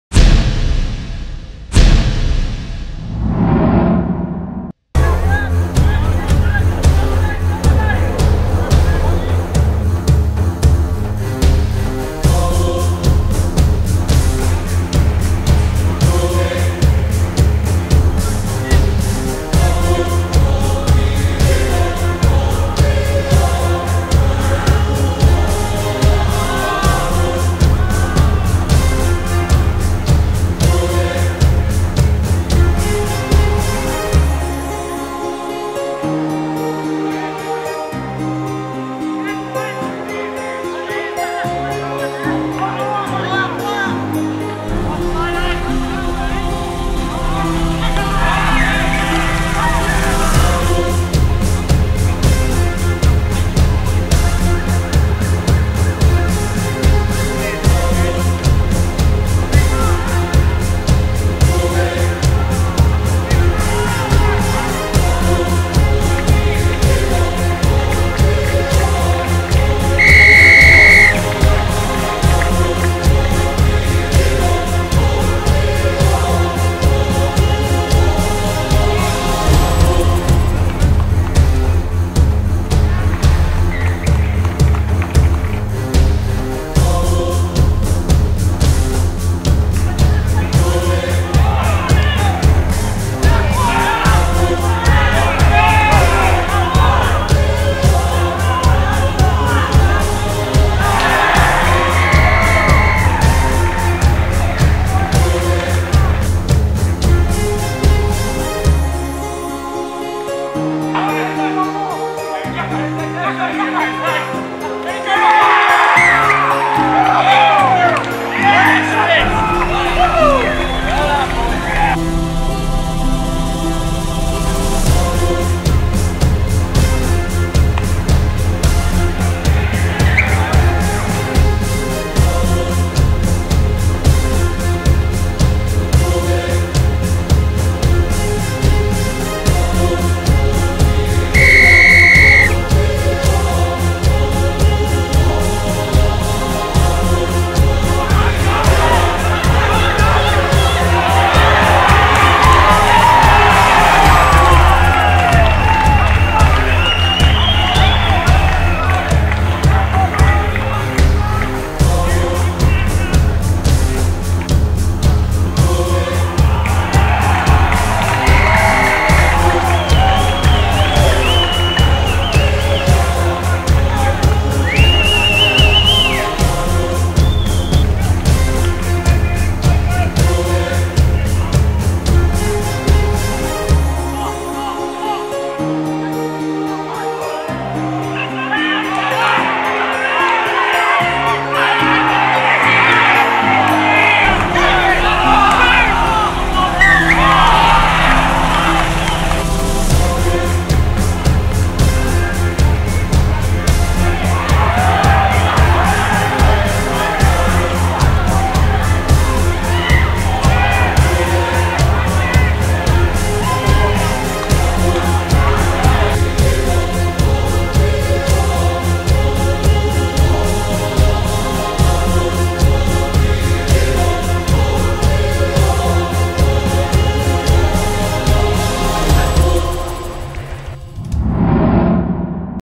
2015 R.07 Colts 1 Randwick V Sydney University
Sydney University 24 Randwick 22